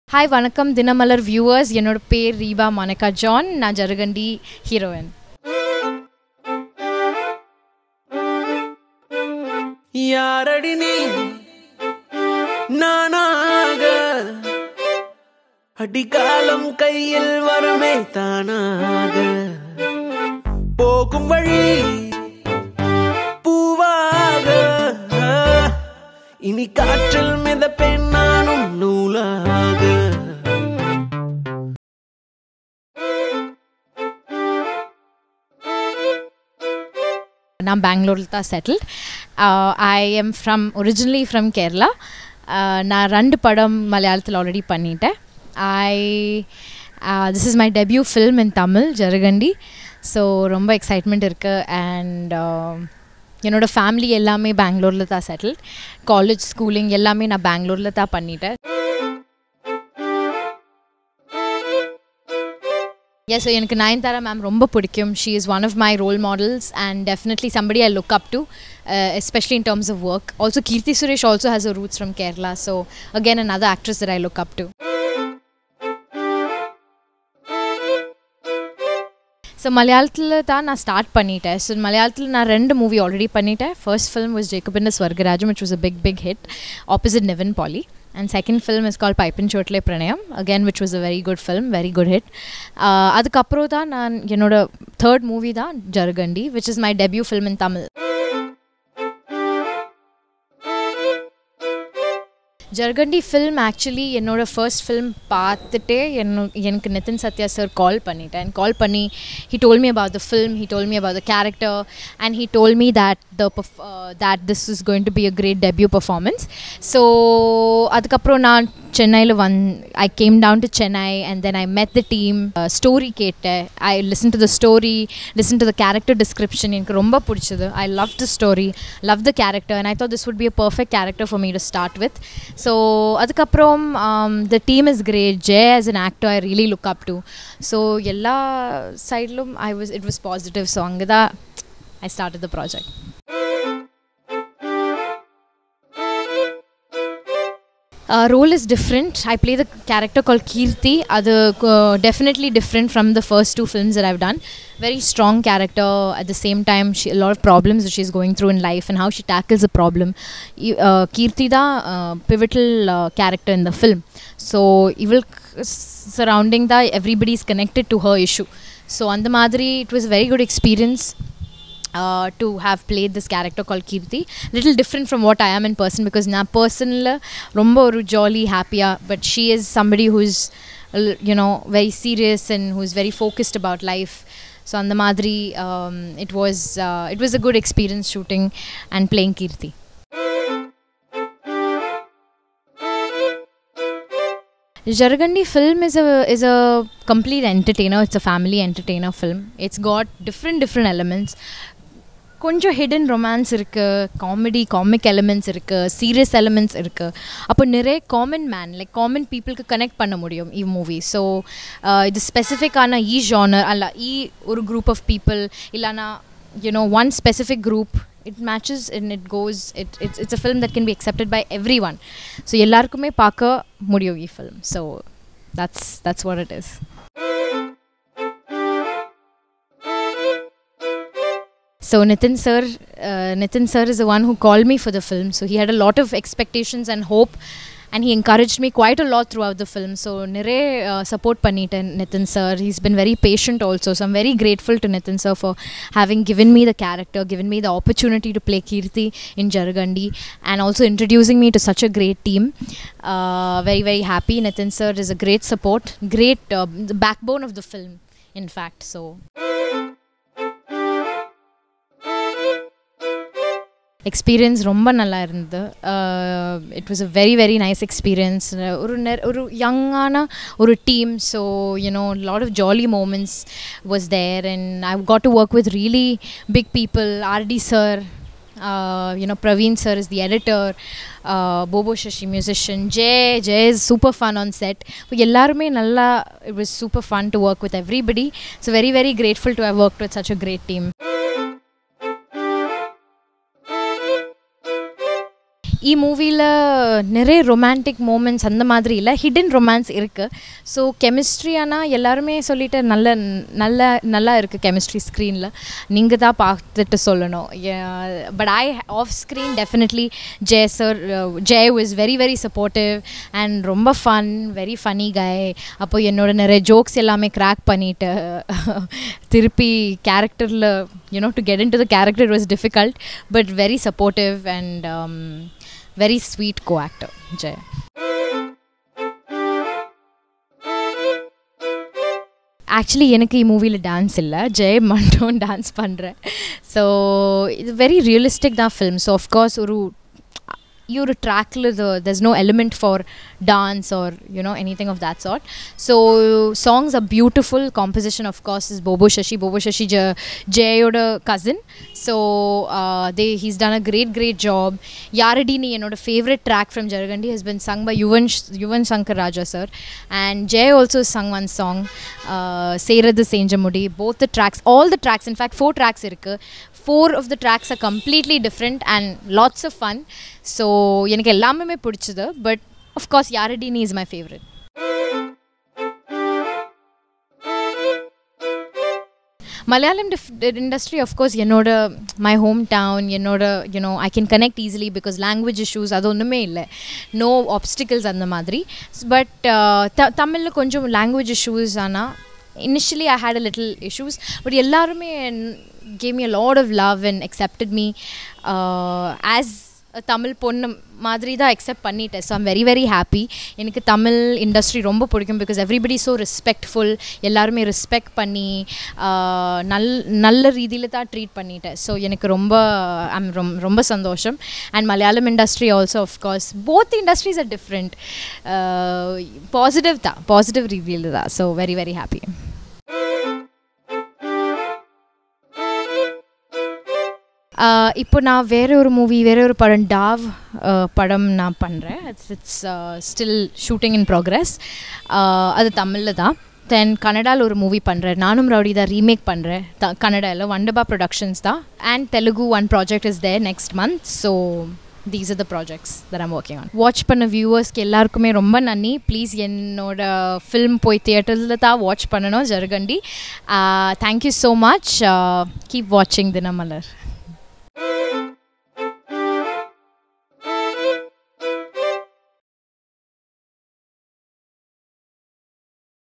தமிழ் சினிமா எனக்கு ரொம்ப பிடிக்கும்: ரெபா மோனிகா ஜான்
தமிழ் சினிமா எனக்கு ரொம்ப பிடிக்கும்: ரெபா மோனிகா ஜான் | Reba Monica John For more videos